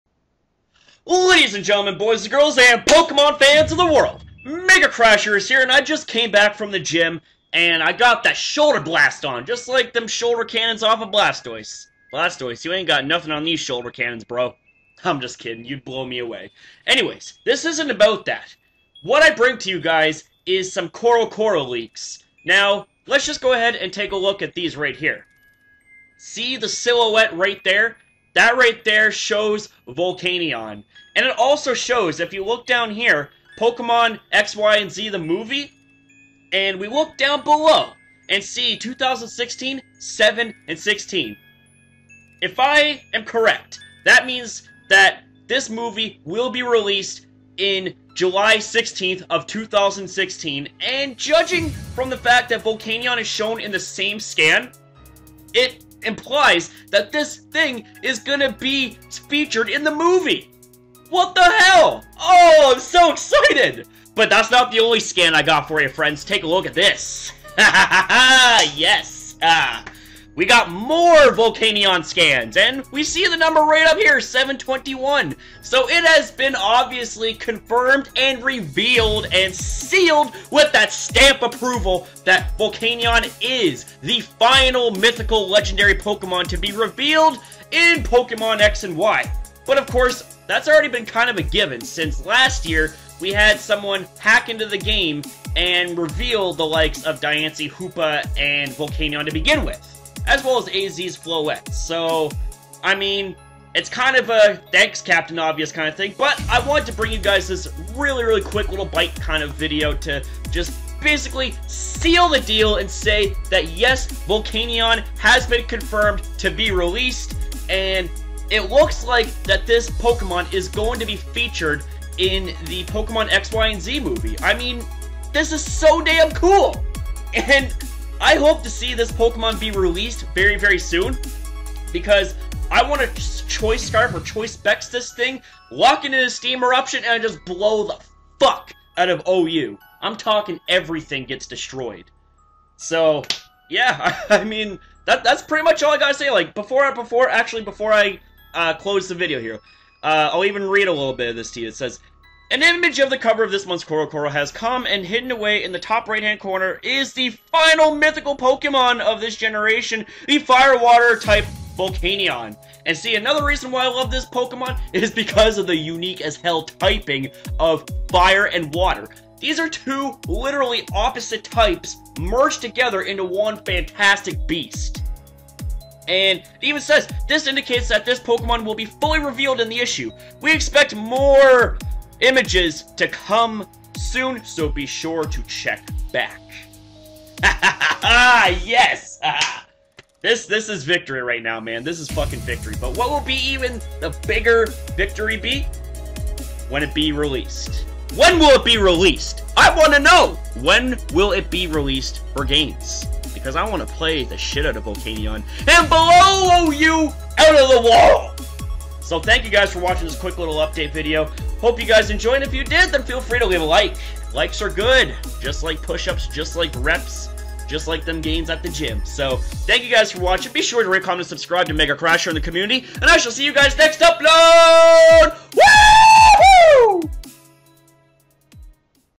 CoroCoro: VOLCANION FINALLY HAS BEEN OFFICIALLY REVEALED!! Pokemon XY&Z Movie Release Date Revealed?
Wow!! Holy chicken fucking strips with fries!! So Volcanion has been officially revealed! On top of that, it looks like the date has also been released for the Pokemon XY&Z movie! :D